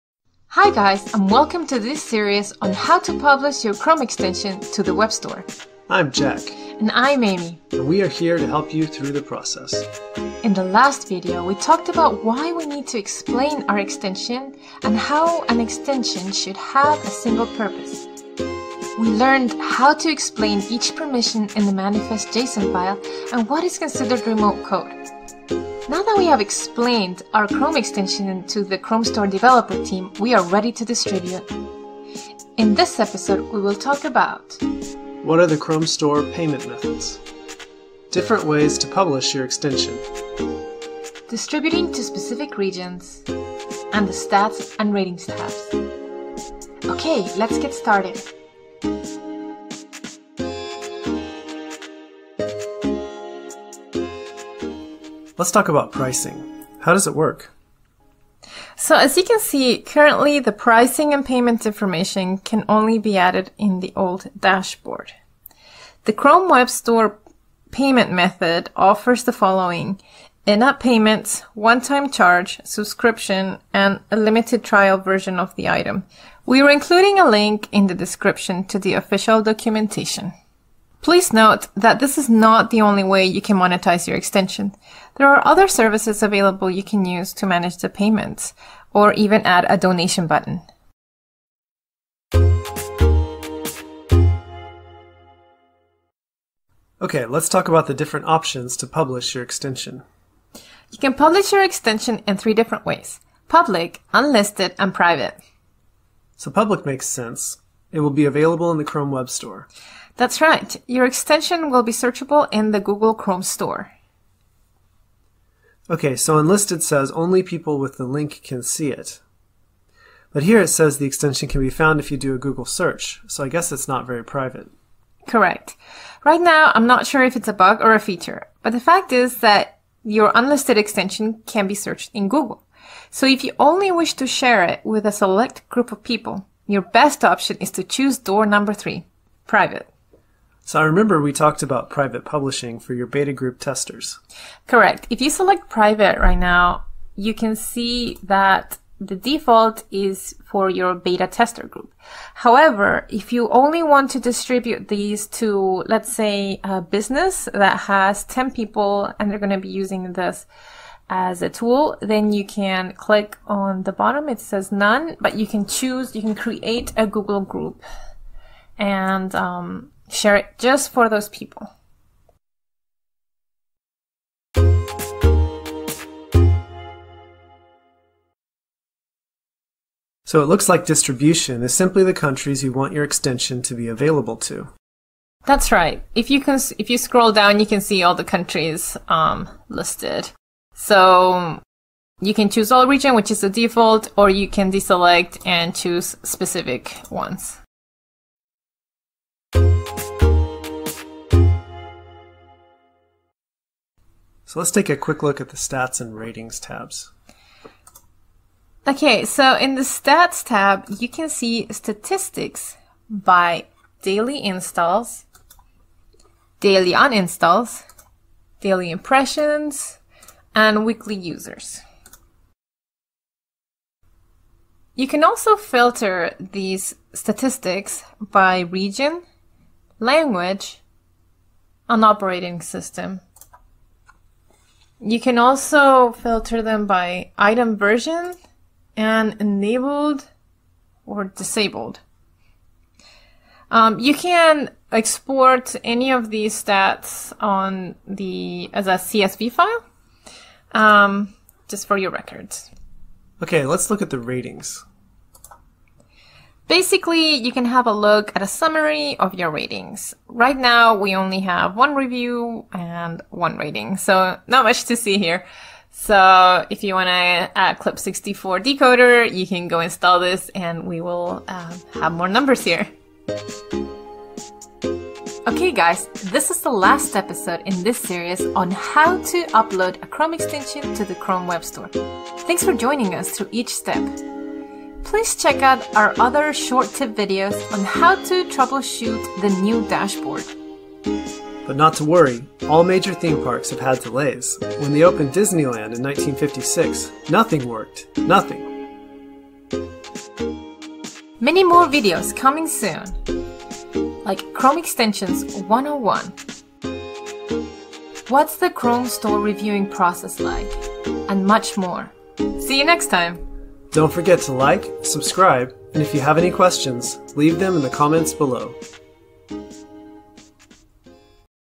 How to Publish your Chrome Extension 4/4 - Distributing
We will show you how to distribute your Chrome Extension on the Chrome Web Store in 2020 using the new Developer Dashboard.

0:00 Introduction
0:50 Chrome Store Payment Options

1:35 Different ways to publish your Chrome Extension
1:55 Publishing an Unlisted Chrome Extension
2:20 Publishing only to a beta testing team

2:31 Distribute only to members of a google group
2:58 Distributing to specific regions
3:20 Statistic reports of your chrome extension
4:13 Overview of your chrome extension ratings

👂 Need a specific help related to chrome extensions? We are all ears!
Leave your requests in the comment section 🙂